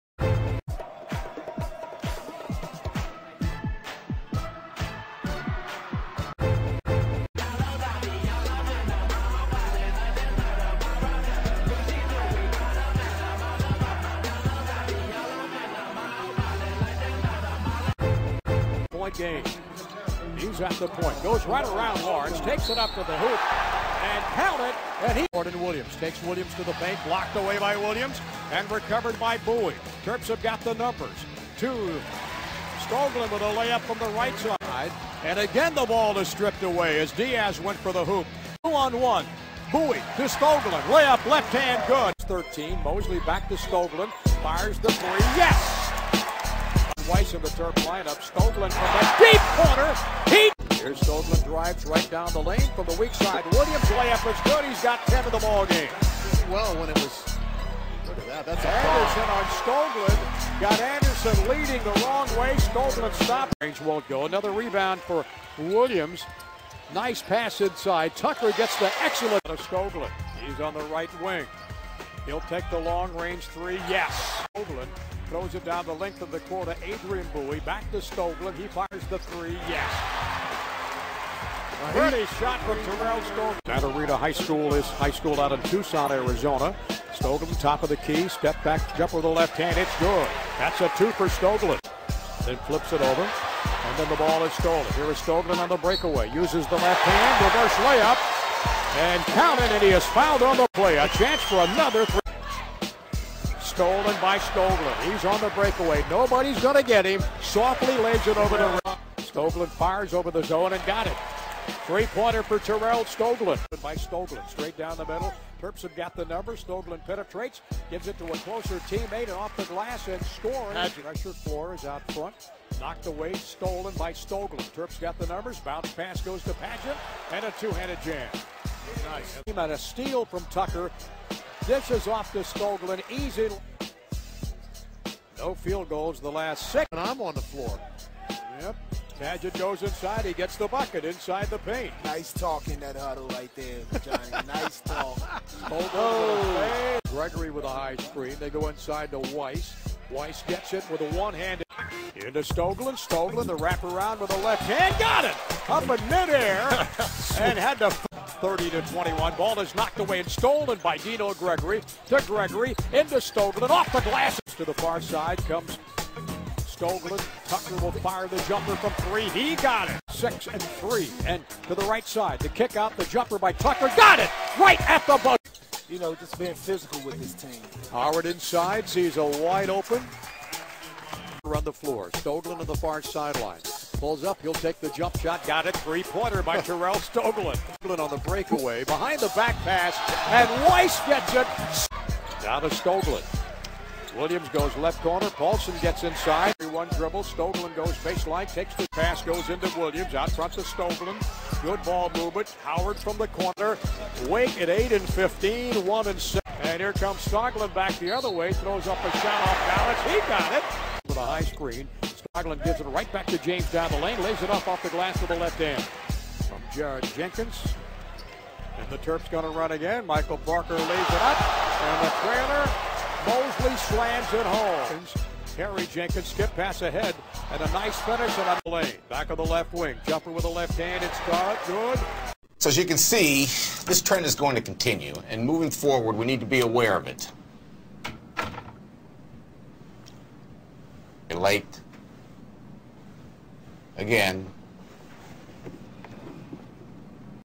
Terell Stoglin 2010-2011 Maryland Terrapin Basketball Highlights
clips from Maryland's point guard Terrell Stoglin

Go TERPS!